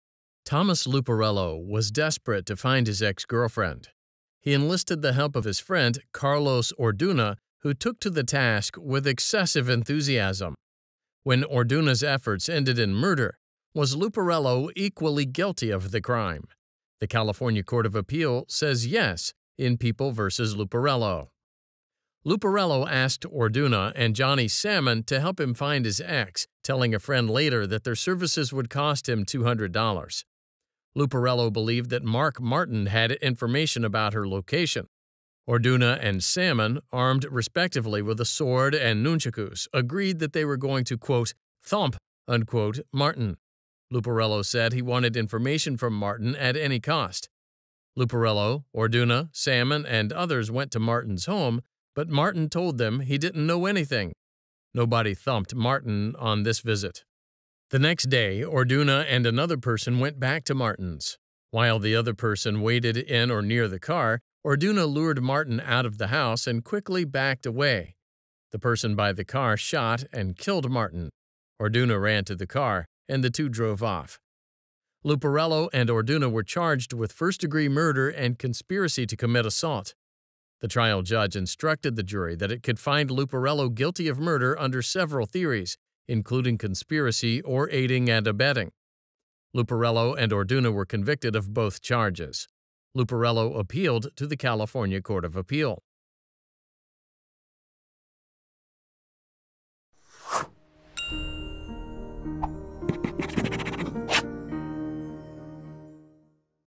People v. Luparello Case Brief Summary | Law Case Explained
People v. Luparello | 231 Cal. Rptr. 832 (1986)

Thomas Luparello was desperate to find his ex-girlfriend. He enlisted the help of his friend, Carlos Orduna, who took to the task with excessive enthusiasm. When Orduna’s efforts ended in murder, was Luparello equally guilty of the crime? The California Court of Appeal says yes, in People versus Luparello.

Luparello asked Orduna and Johnny Salmon to help him find his ex, telling a friend later that their services would cost him $200. Luparello believed that Mark Martin had information about her location. Orduna and Salmon, armed respectively with a sword and nunchakus, agreed that they were going to, quote, “thump,” unquote, Martin. Luparello said he wanted information from Martin at any cost. Luparello, Orduna, Salmon, and others went to Martin’s home, but Martin told them he didn’t know anything. Nobody thumped Martin on this visit.

The next day, Orduna and another person went back to Martin’s. While the other person waited in or near the car, Orduna lured Martin out of the house and quickly backed away. The person by the car shot and killed Martin. Orduna ran to the car, and the two drove off.

Luparello and Orduna were charged with first-degree murder and conspiracy to commit assault. The trial judge instructed the jury that it could find Luparello guilty of murder under several theories, including conspiracy or aiding and abetting. Luparello and Orduna were convicted of both charges. Luparello appealed to the California Court of Appeal.

Want more details on this case? Get the rule of law, issues, holding and reasonings, and more case facts

Have Questions about this Case? Submit your questions and get answers from a real attorney